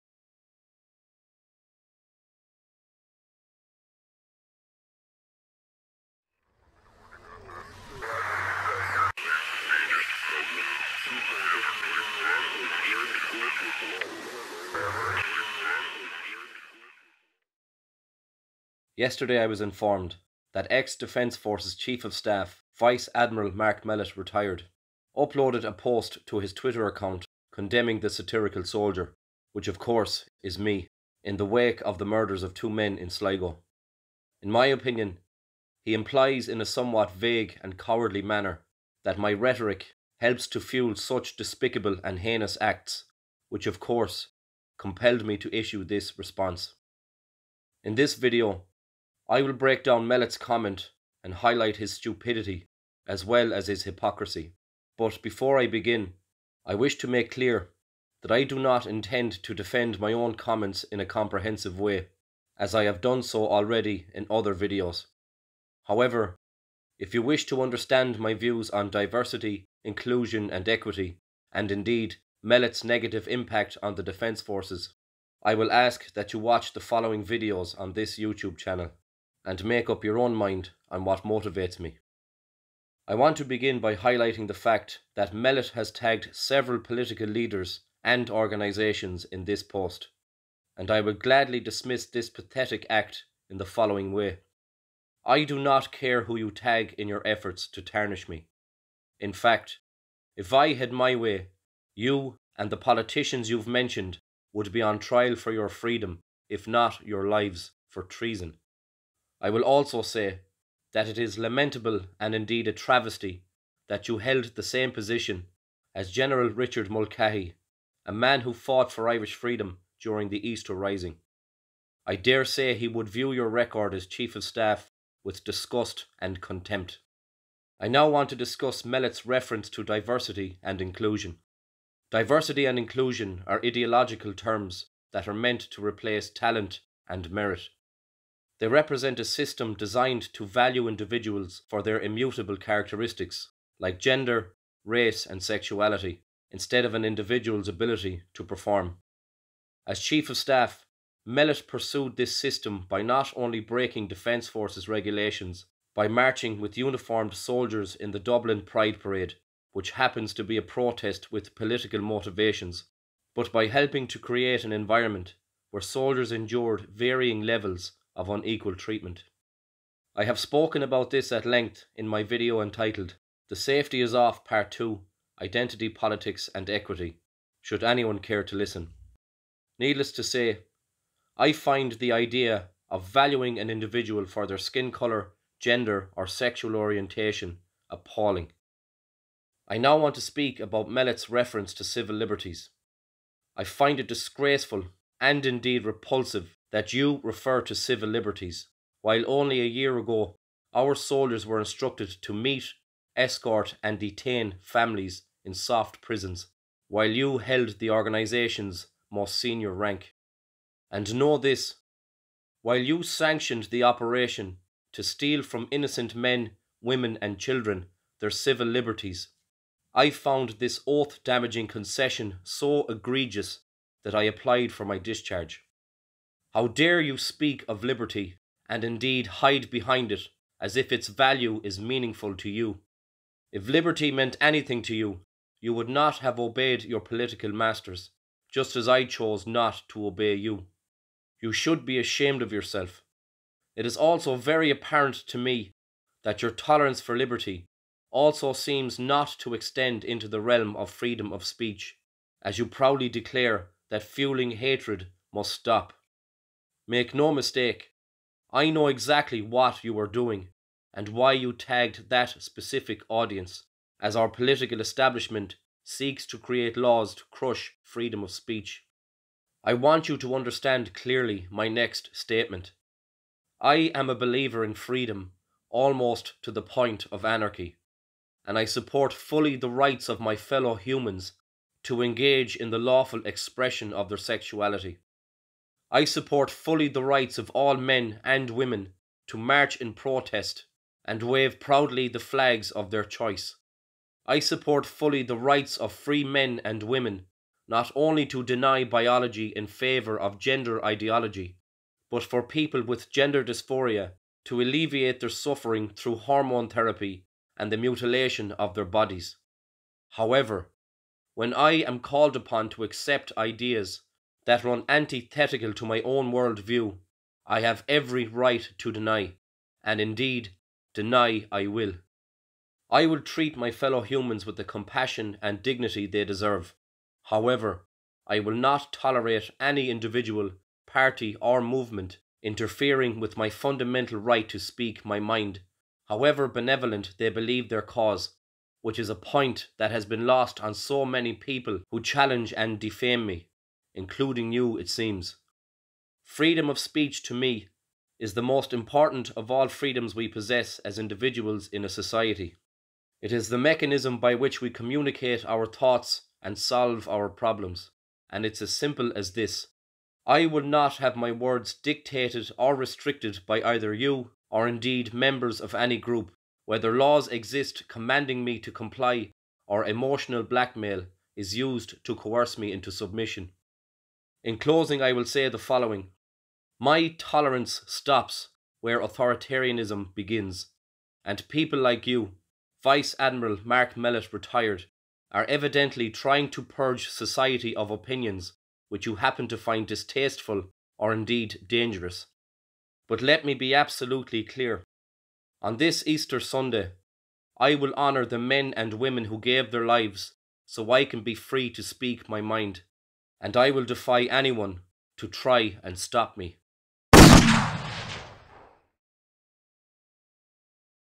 My Response To Vice Admiral Mellett (Retired)
Ex-Defence Forces Cheif Of Staff Vice Admiral Mark Mellett (Retired) created a Twitter post referring to Satirical Soldier. This video is my response.